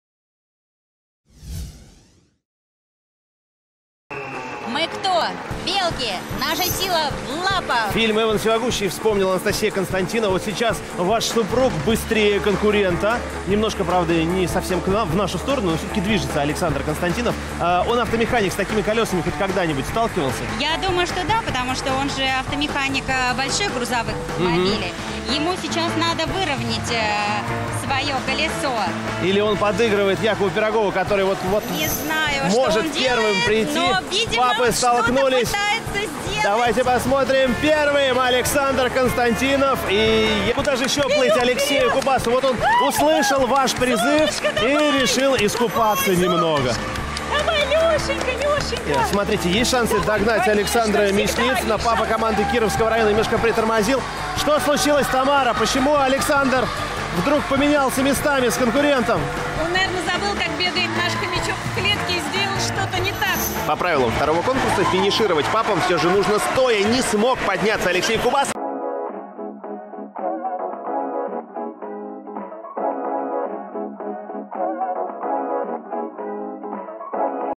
Надувной аттракцион Беличье колесо
Суть водного аттракциона Беличье колесо в том, что вам нужно пройти необходимую дистанцию внутри колеса на скорость.

Попав в огромное колесо, вы передвигаетесь за счет собственной моторики рук и ног, перескакивая по надувным баллонам, таким образом, надувное колесо разгоняется и увеличивает скорость.